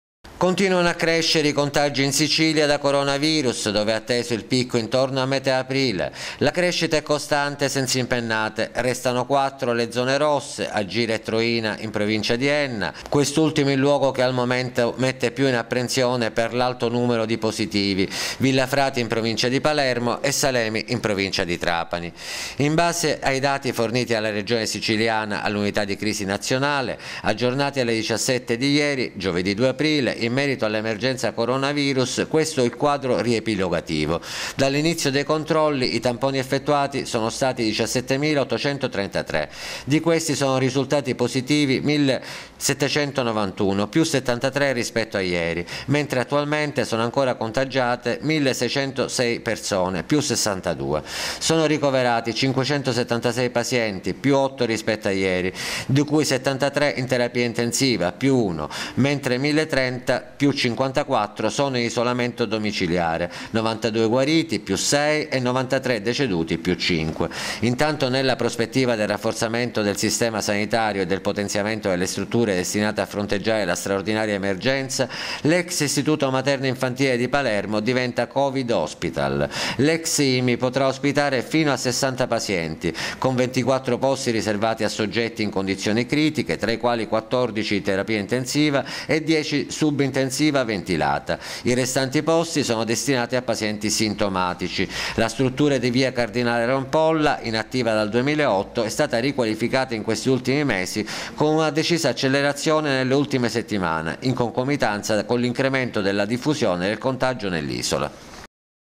Palermo. In Sicilia continuano a crescere i contagi
Continuano a crescere i contagi in Sicilia  da Coronavirus, dove è atteso il picco intorno a metà aprile. La crescita è costante senza impennate. Restano 4 le zone rosse Agira e Troina in provincia di Enna, quest’ultimo il luogo  che al momento mette piu’apprensione per l’alto numero di positivi, Villafrati in provincia di Palermo e Salemi in provincia di Trapani.